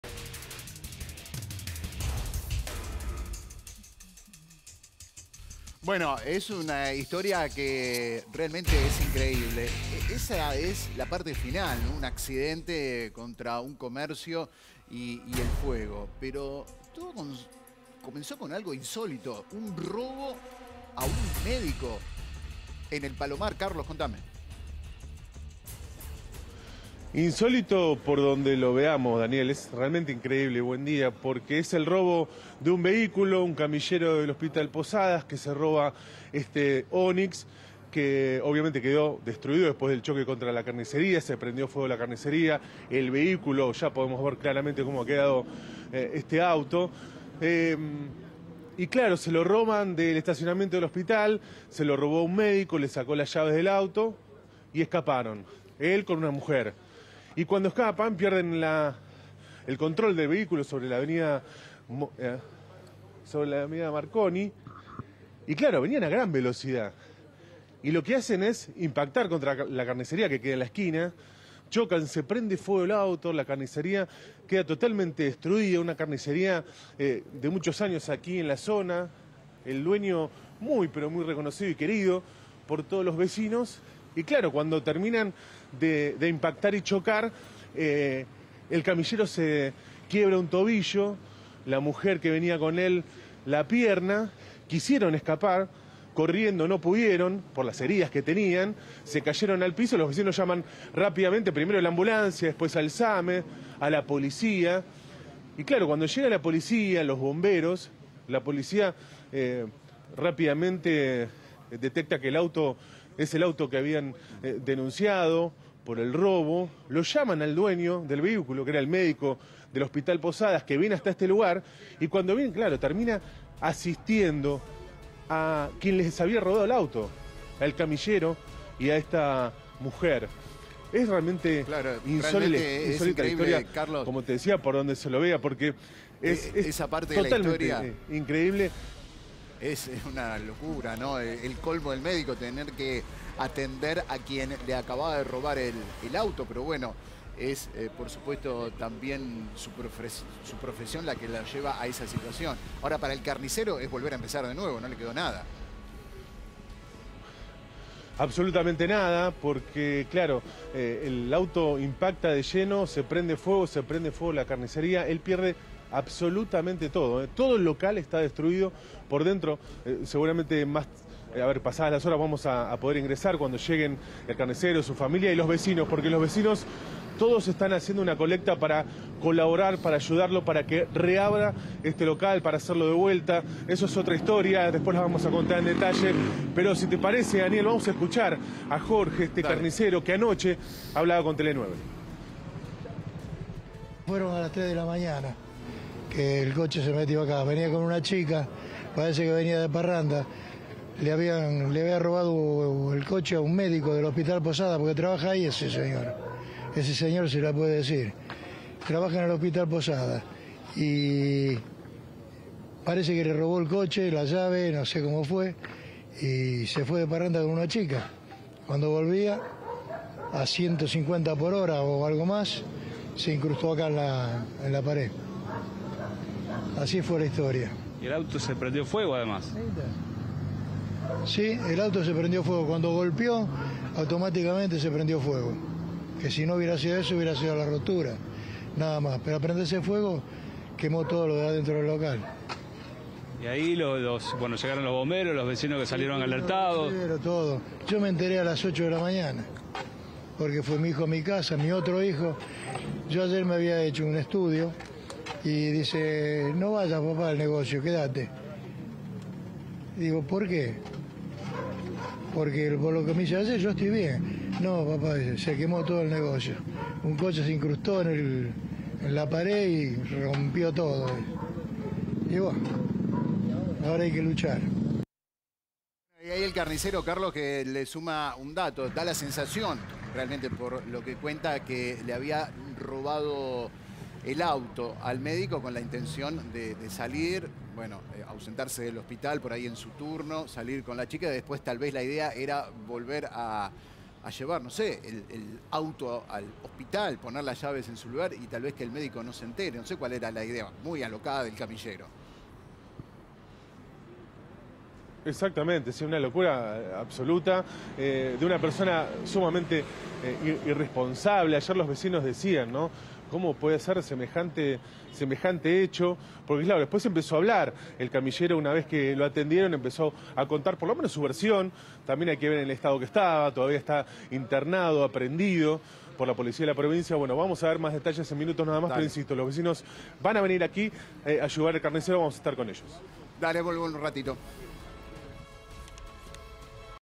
De no creer: un camillero le robo el auto a un médico, chocó y tuvo que ser asistido por él
El insólito incidente se desarrolló cuando el ladrón, que trabajaba en el hospital Posadas, se apoderó del vehículo del profesional de la salud, y terminó teniendo un siniestro poco después. La victima no dudó en asistir a los heridos, que incluían al delincuente y su acompañante.